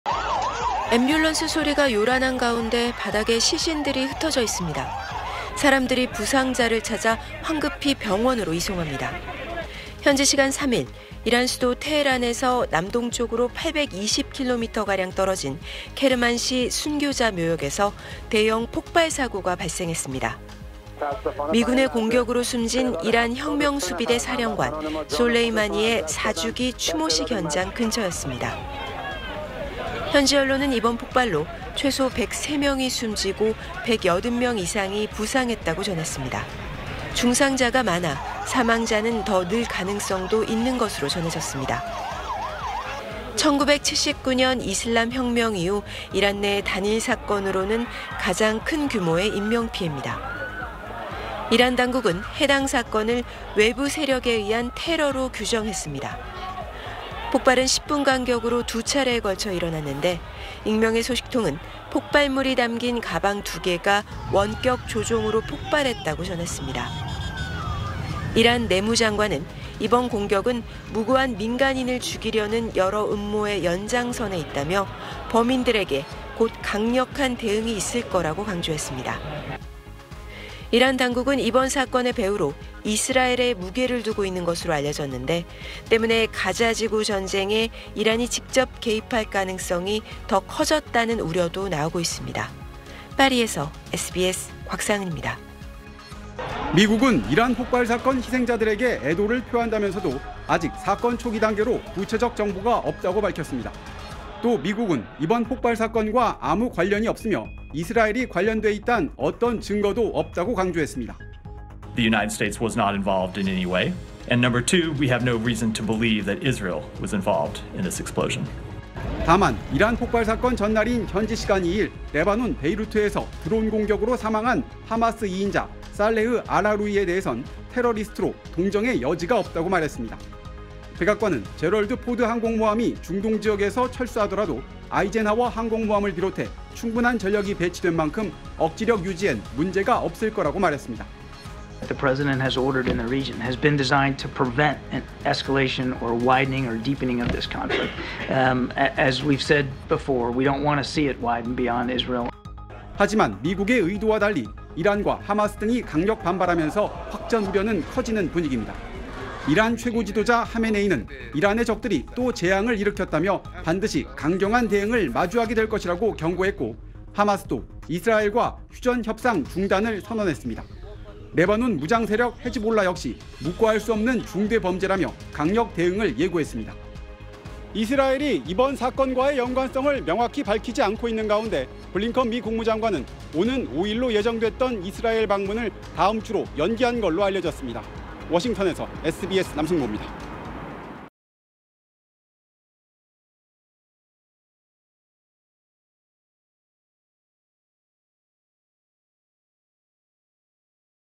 이란 추모식에서 두 번 터진 의문의 폭발..미국 "이스라엘도, 우리도 아니다" / SBS / 모아보는 뉴스
00:00 이란 사령관 추모식 대형 폭발 사고…"최소 103명 사망"
01:36 "이란 폭발 사고 미국과 무관…이스라엘 연루 근거 없어"